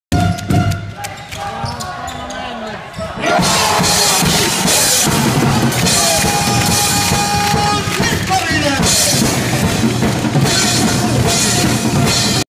Antto Nikkarinen vie Bisonsin kolmosella 9-6-johtoon
Korisliigan ottelu Bisons Loimaa vs. Helsinki Seagulls Loimaalla 3.4.2015. Bisons voitti 85-70. Antto Nikkarinen teki kolmosella tilanteeksi 9-6.